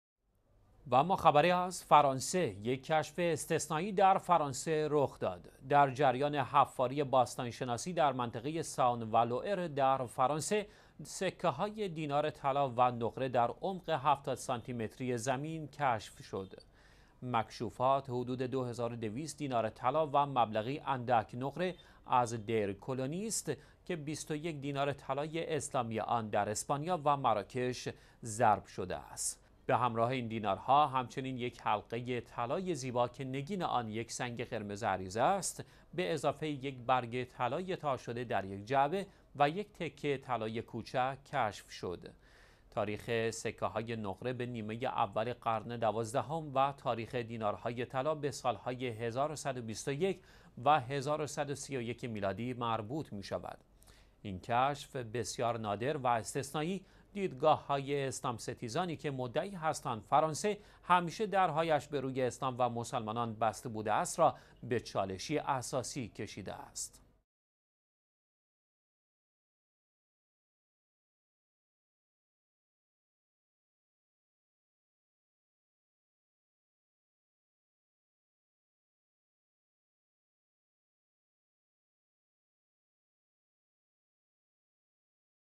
دینارهای طلای اسلامی در فرانسه کشف شد
یک کشف استثنایی در فرانسه رخ داد.

در جریان حفاری باستان شناسی در منطقه ساون و لوئر، یکی از مناطق فرانسه، سکه های دینار طلا و نقره در عمق 70 سانتی متری زیر زمین در محل حفاری کشف شد.

مکشوفات حدود 2200 دینار طلا و مبلغ بسیار ناچیزی نقره از دیر کلونی است که 21 دینار طلای اسلامی آن در اسپانیا و در مراکش ضرب شده است.

به همراه این دینارها، همچنین یک حلقه طلای زیبا که نگین آن یک سنگ قرمز عریض است به همراه  یک برگ طلای تا شده در یک جعبه و یک تکه طلای کوچک کشف شد.

تاریخ این سکه ها مربوط به نیمه اول قرن دوازدهم برای دینارهای نقره و بین سال های 1121 و 1131 برای دینارها طلا می باشد.

این کشف بسیار نادر و کاملا استثنایی، اسلام ستیزانی که مدعی هستند فرانسه همیشه درب هایش به روی اسلام و مسلمانان بسته بوده است، را به چالشی اساسی کشیده است.

شنبه - ۲۷ آبان ۱۳۹۶
السبت - ۲۸ صفر ١٤٣٩